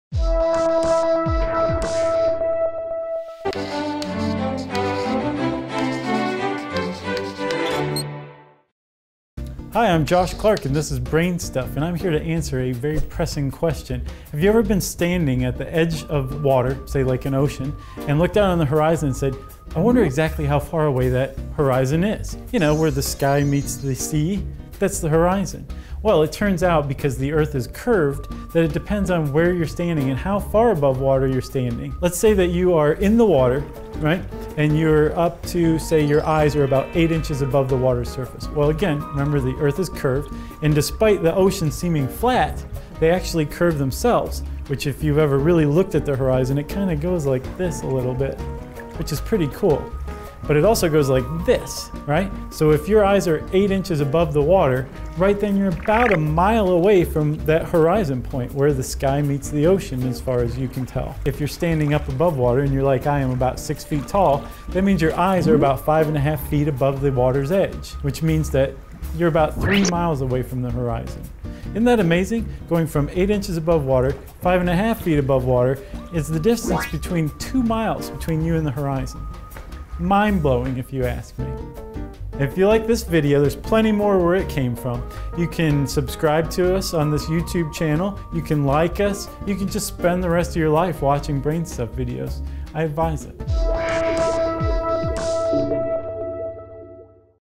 How Does The Horizon Work?
When you stand at the water's edge and look out toward the horizon, how far away can you actually see? Check out this episode of BrainStuff to learn more about the horizon and the curvature of Earth.